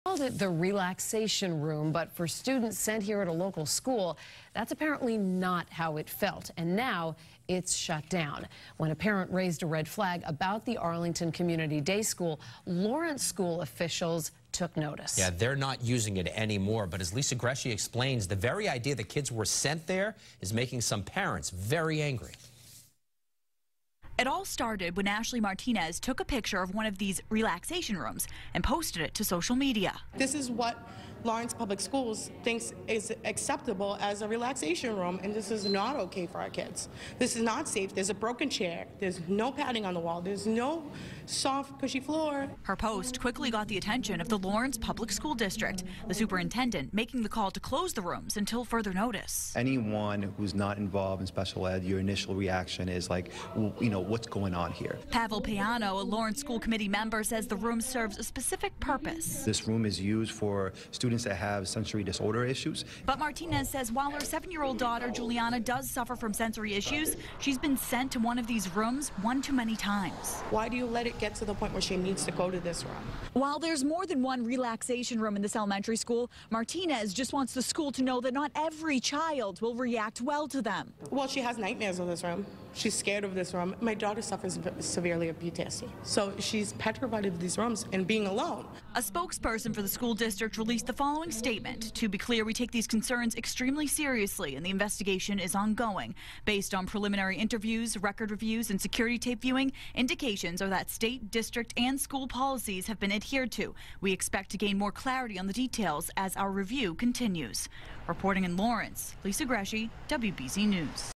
Student Has Nightmares From 'Relaxation Room' At Lawrence School
A mother says her 7-year-old daughter has nightmares because she has been sent to a relaxation room at school. WBZ-TV's Lisa Gresci reports.